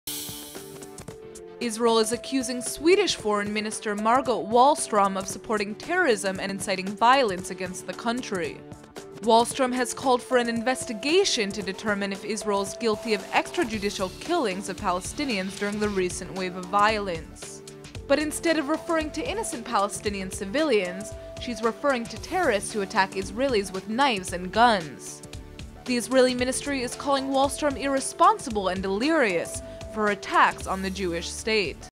Swedish Foreign Minister Seems to Support Terrorism
Does Foreign Minister Margot Wallstrom seems to support Palestinian terror.